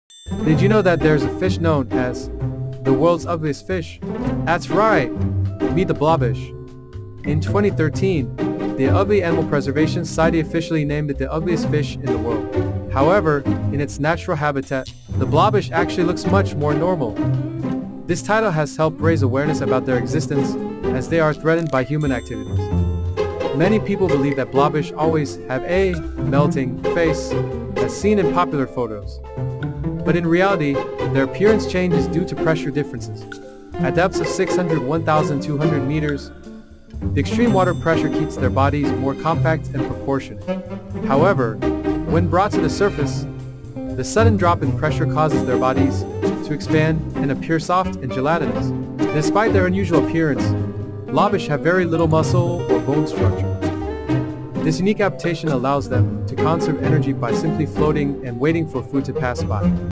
the world's ugliest fish
Did you know that there's a fish known as "the world's ugliest fish"?  Meet the blobfish! In 2013, the Ugly Animal Preservation Society officially named it the ugliest fish in the world. But guess what? In its natural deep-sea habitat at depths of 600–1,200 meters, the blobfish actually looks much more normal!
Its "melting" face is just a result of pressure changes when brought to the surface. Blobfish have very little muscle or bone structure, allowing them to float effortlessly and conserve energy in the deep-sea environment.
Even though it's called ugly, the blobfish is an incredible example of deep-sea adaptation! What do you think — is it really ugly or kind of cute?